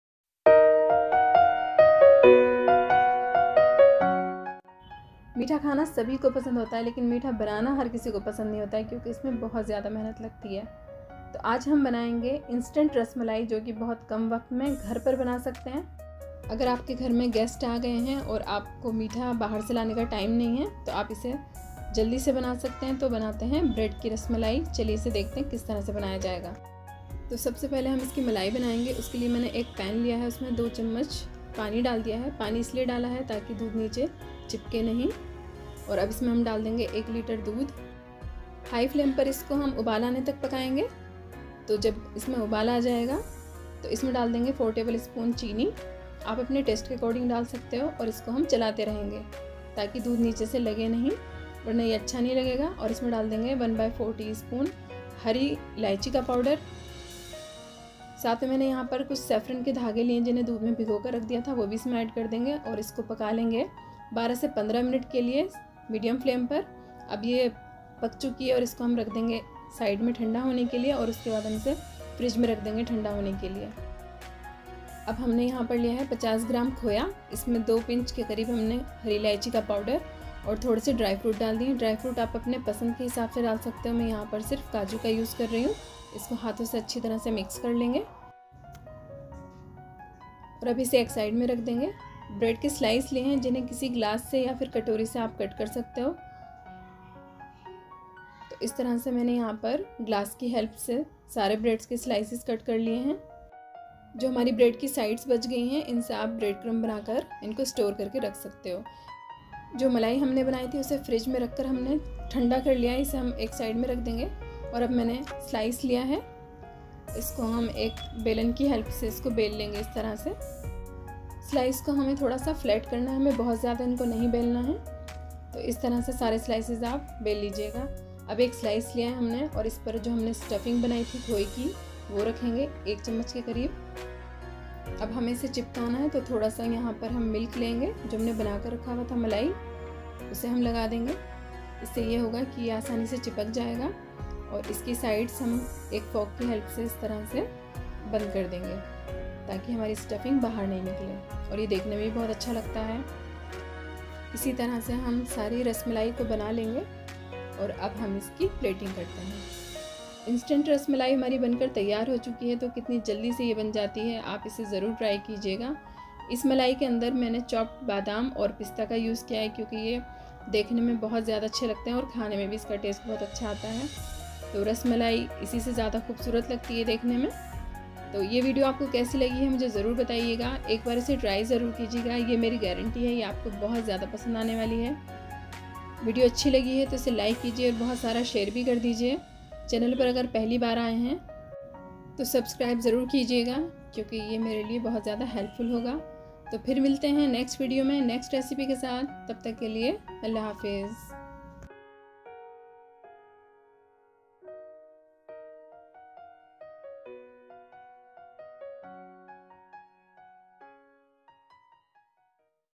Instant Bread Rasmalai now at home! | Easy, Quick and delicious | Food 4 Family
Now make Rasmalai at home with just a few ingredients and within a few minutes!!
We are happy to share this quick and easy reciper with all of you and we hope that you enjoy our content, for any queries and issues, drop a comment!
xoxo~